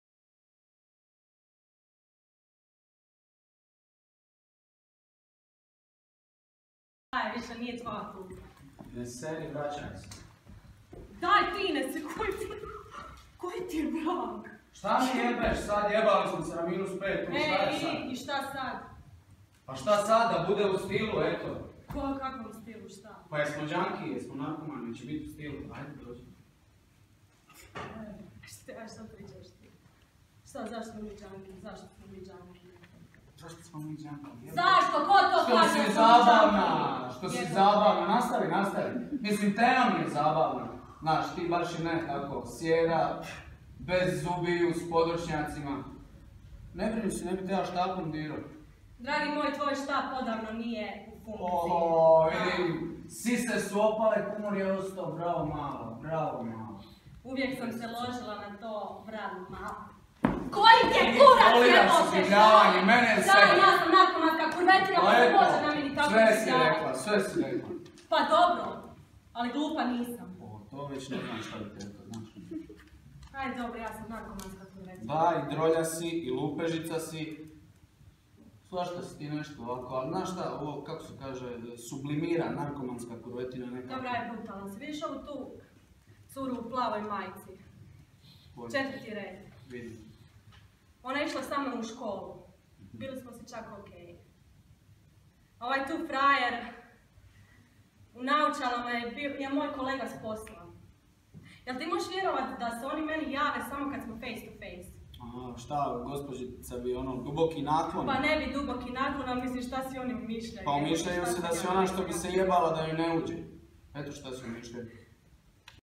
Melita, Karmen, labudica… kako vam drago
Ivana Mijić Vulinović „Melita, Karmen, labudica… kako vam drago“
Redatelj: Christian Jalzecic
Igraju: Josipa Anković, Ivan Pašalić
(ulomak)
3. dani Ogranka mladih HDDU-a
Klupa u parku, 21. travnja 2017., GDK "Gavella"